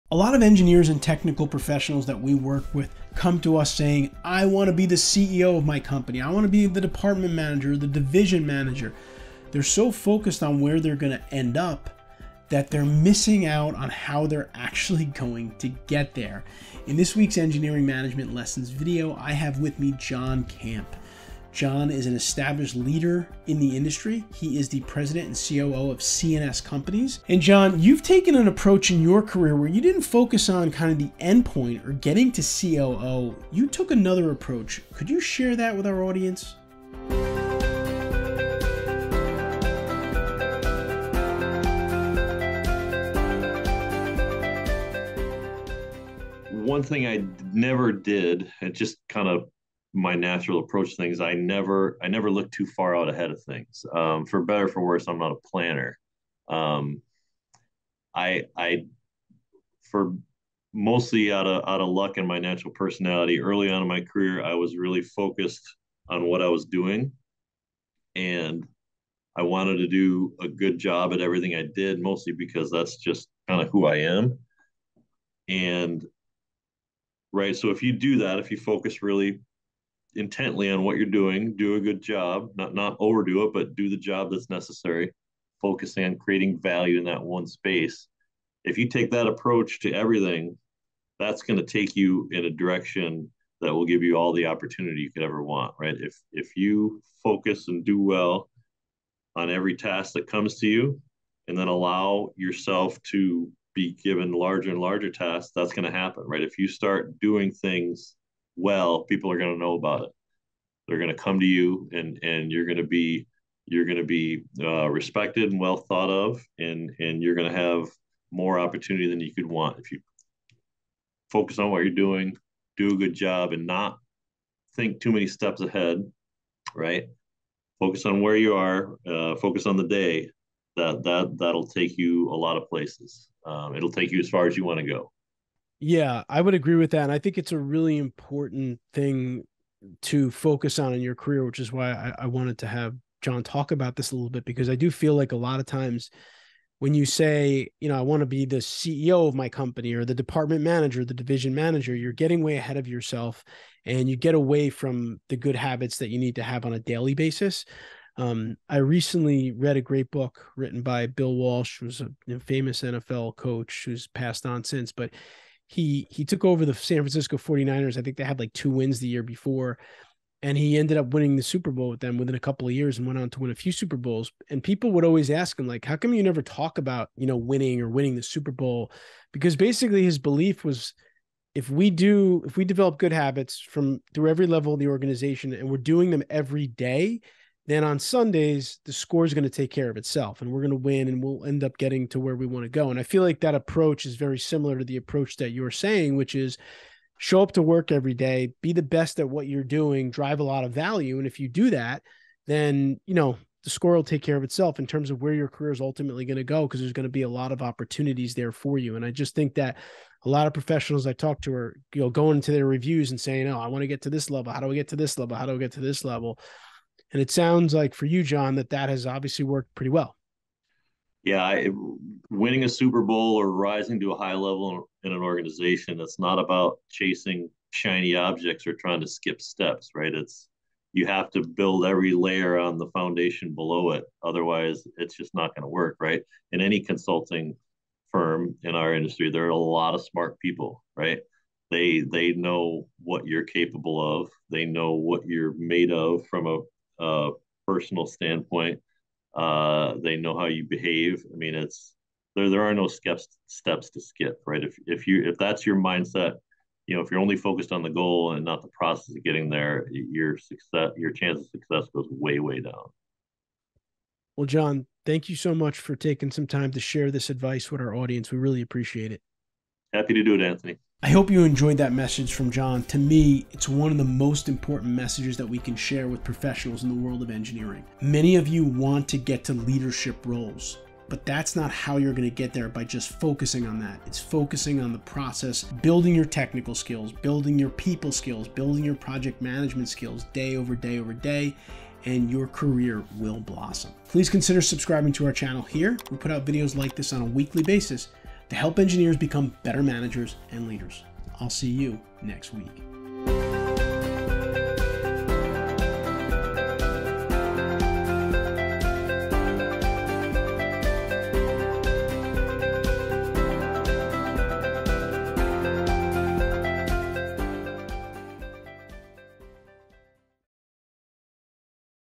Want To Achieve Your Goals? Then Don’t Focus on Them | Do This Instead!
In this video, John Camp, P.E., President & COO at C&S Companies talks about the significance of cultivating good habits in your engineering career. Learn why it's crucial not to rush your career growth and how daily good practices can pave the way to greater accomplishments. Unlock the Secret to Goal-Setting